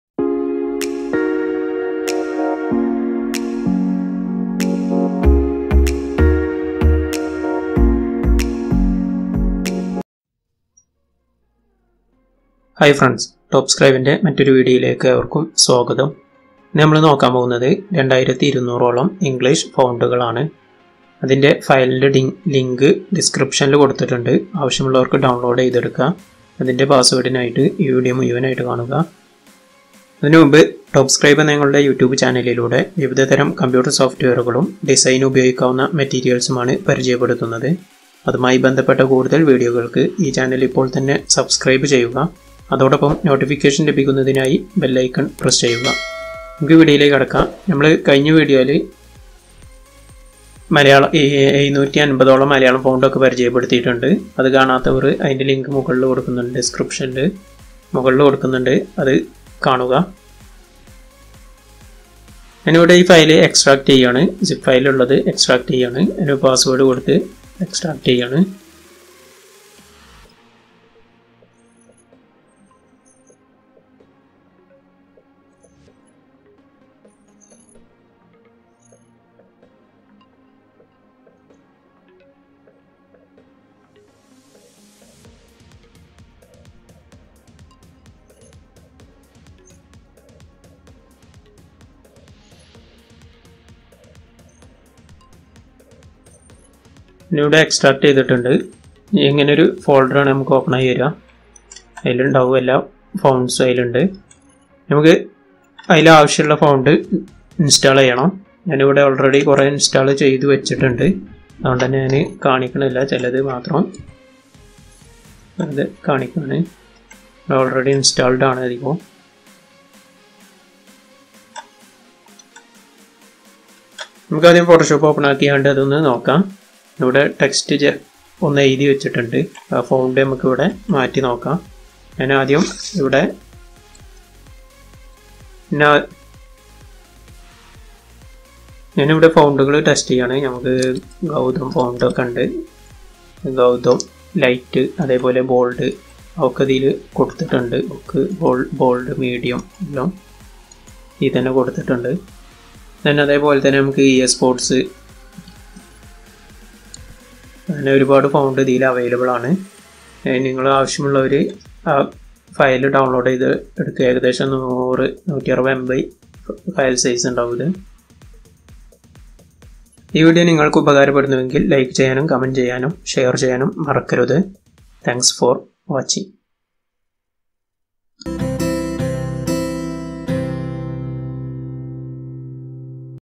2200+ English Fonts Download and Installation Malayalam
how to download and install the 2200 and more than english fonts in malayalam, it very helpful to designers and data entry workers please watch the video fully for getting the password